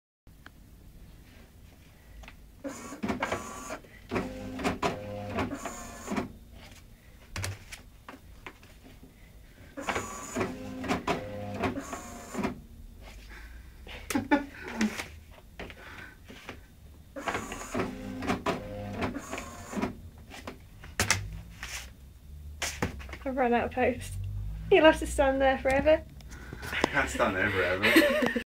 Funny Robot man with sound effects - franking machine
Geoff being a robot with sound effects by the franking machine!!